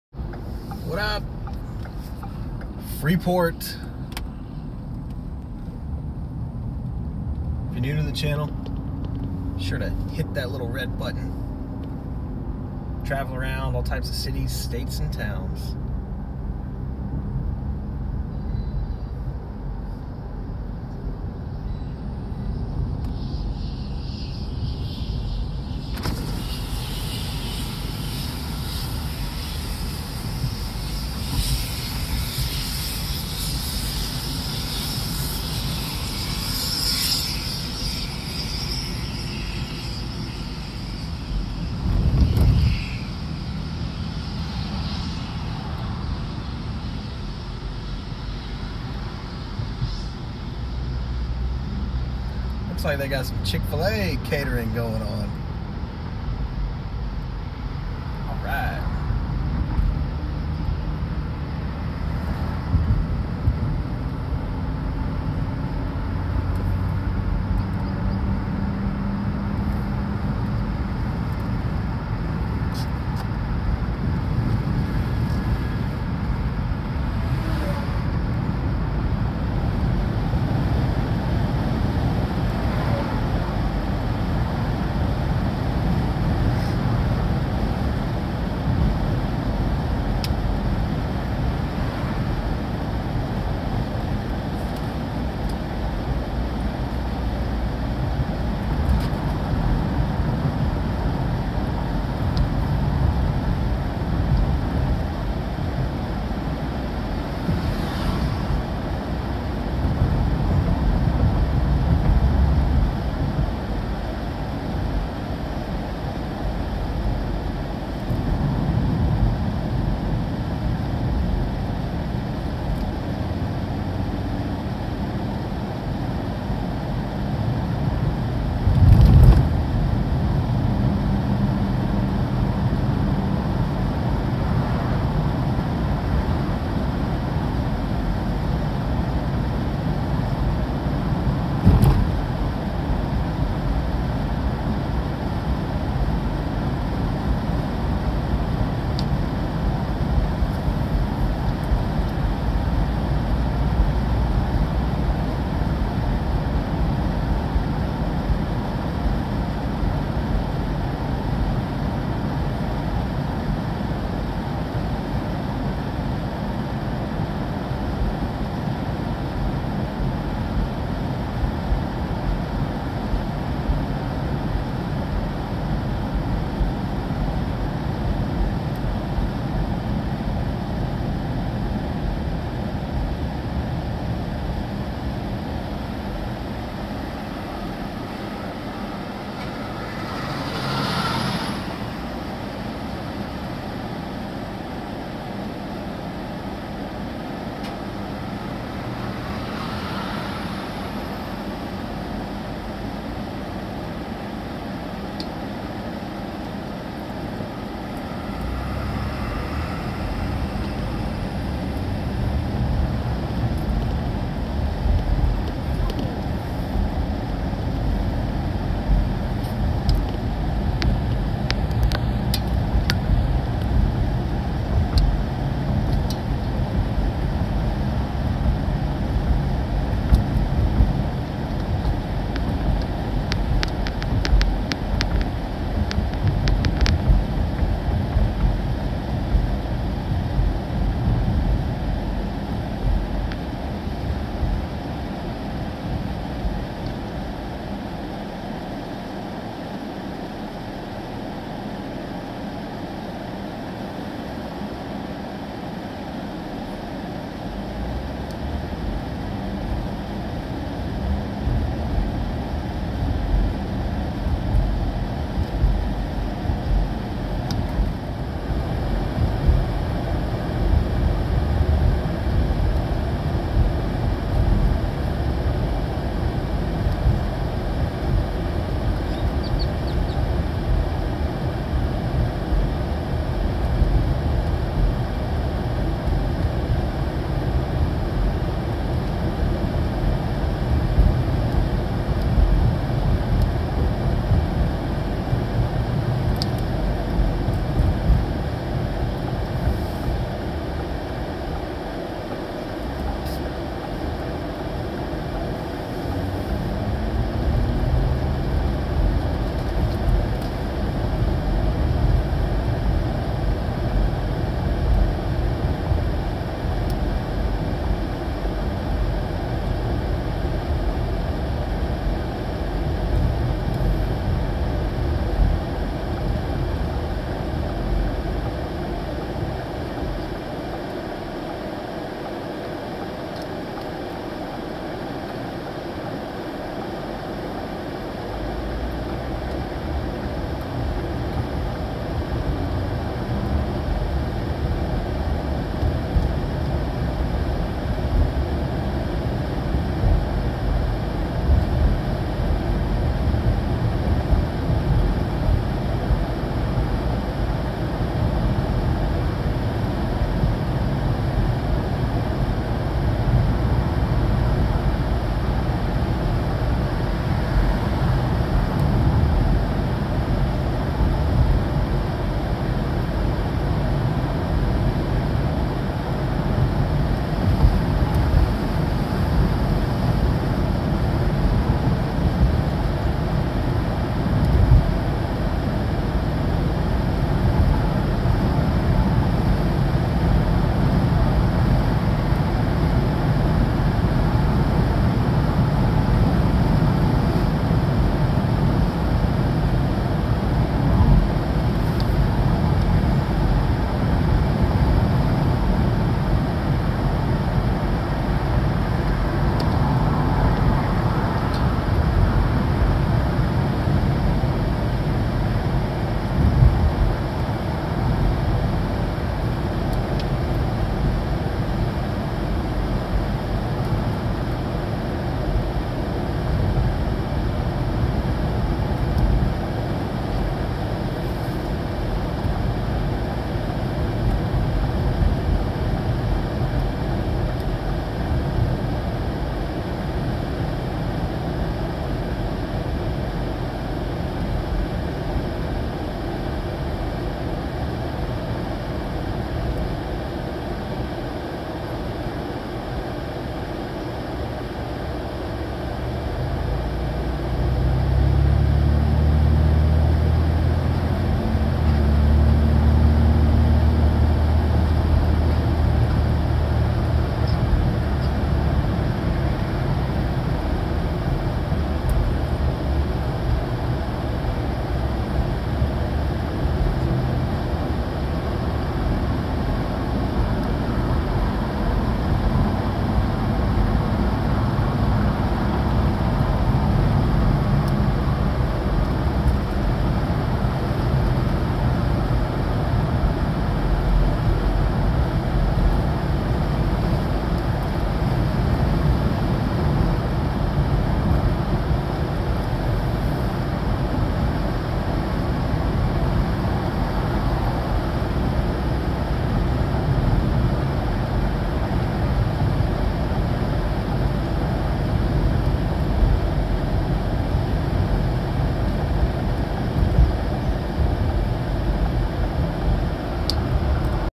Freeport, Texas - Driving Around The City
Taking a trip down to the Texas Coast Line and visiting the city of Freeport. This South Houston city is home to many chemical and refinery plants. I must say, the surf next at Surfside Beach is always a foot or so higher than Galveston.

Come along and let's roll!